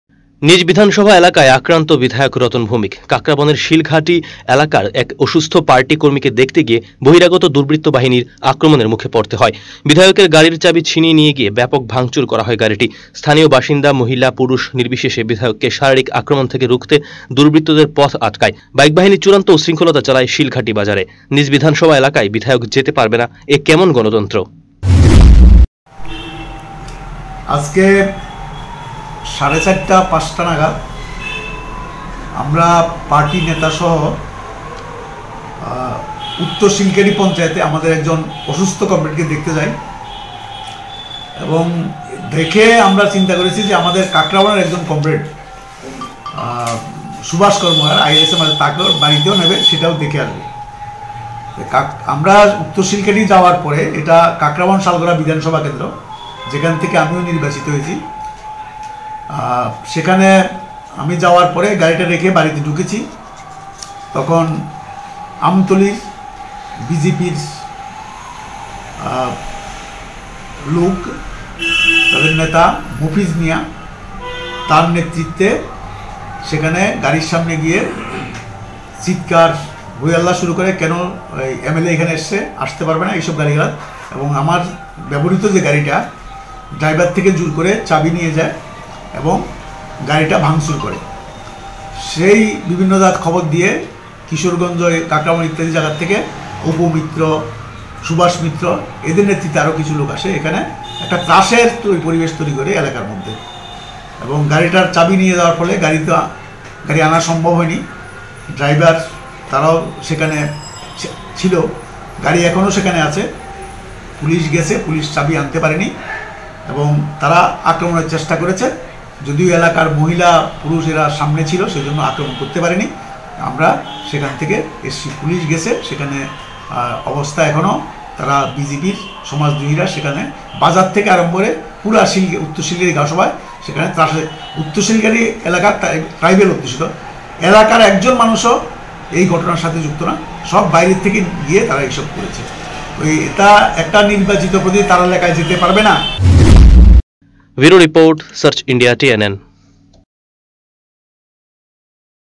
আক্রান্ত বিধায়ক রতন ভৌমিক
নিজের বিধান সভা এলাকাতেই দুস্কৃতিদের হাতে আক্রান্ত বাম বিধায়ক রতন ভৌমিক। মঙ্গলবার বিকালে শিলঘাটি এলাকার এক অসুস্থ পার্টি কর্মীকে দেখতে গিয়েছিলেন তিনি। সেখানেই উনার গাড়িতে আক্রমন করে বাইক বাহিনী।